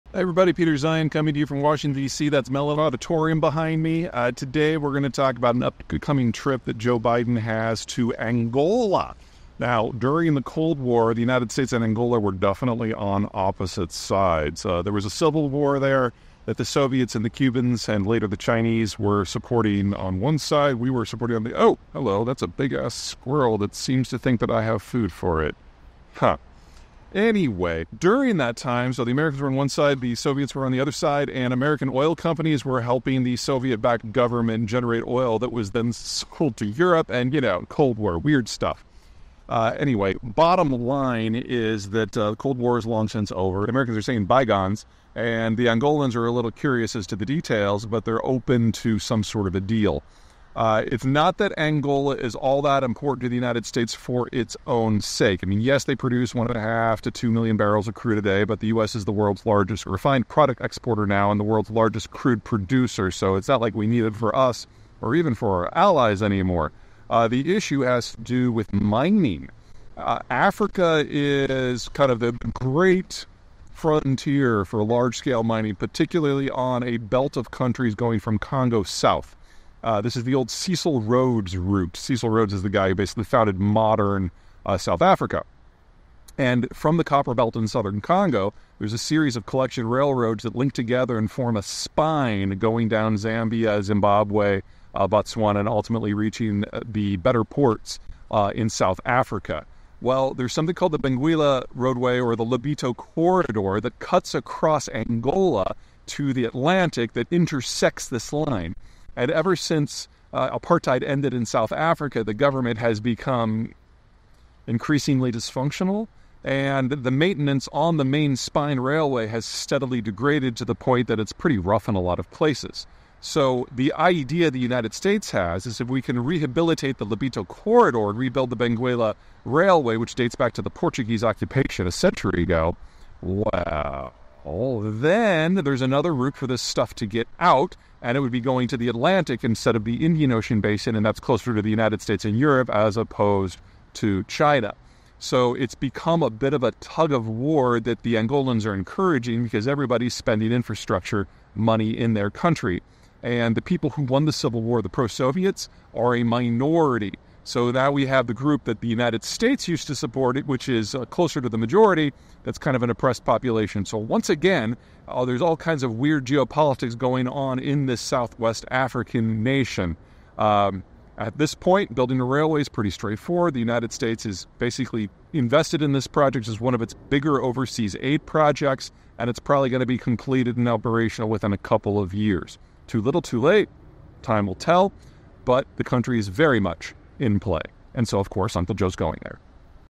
Joe Biden Finally Heads to Angola... (Video Repost) || Peter Zeihan
US President Joe Biden has embarked on the first—and only—sub-Saharan African visit of his presidential term.